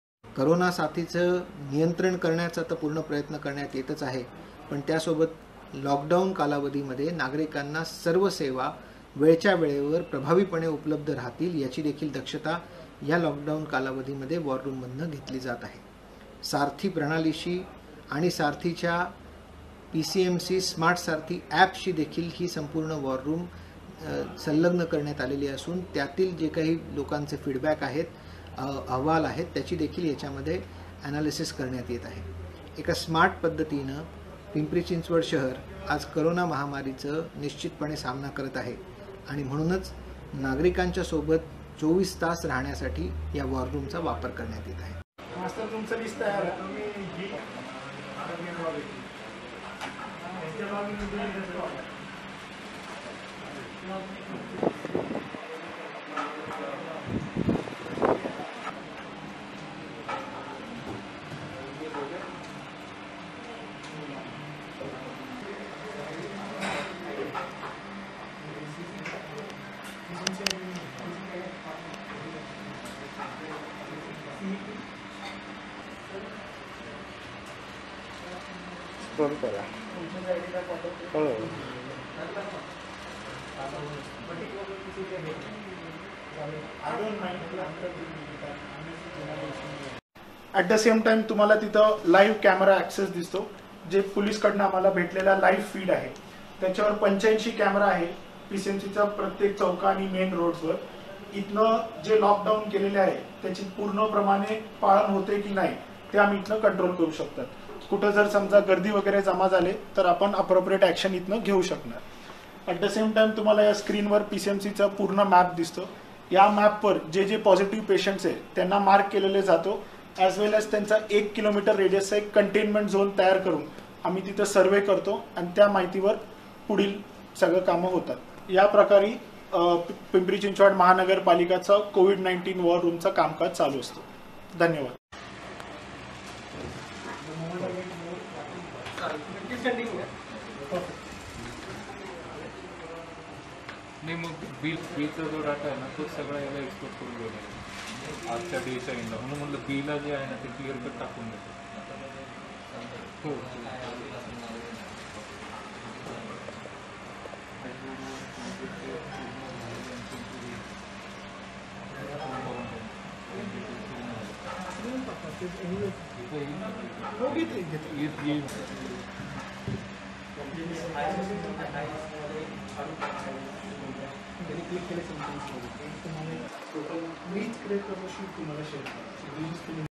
पिंपरी चिंचवडमध्ये लॉकडाउन काळात वॉर रूम द्वारे घेतली जातीये दक्षता – श्रावण हर्डीकर
दिवसेंदिवस कोरोनाच्या रुग्णांना मध्ये वाढ होतीये. कोरोनाशी लढण्यासाठी अनेक ठिकाणी आधुनिक तंत्रज्ञाचा वापर केला जातोय. पिंपरी चिंचवड मध्ये देखील कोरोनाचा प्रादुर्भाव रोखण्यासाठी वॉर रूम सुरु केलीये. यामार्फत शहरातील सर्व ठिकाणी लक्ष ठेवलं जातंय. लॉक डाउनच्या काळात शहरातील नागरिकांना सर्व सोयी सुविधा वेळेवर मिळाव्यात यासाठी वॉर रूम द्वारे विशेष दक्षता घेण्यात येतं असल्याची माहिती पिंपरी चिंचवड चे महापालिका आयुक्त श्रावण हर्डीकर यांनी दिलीये.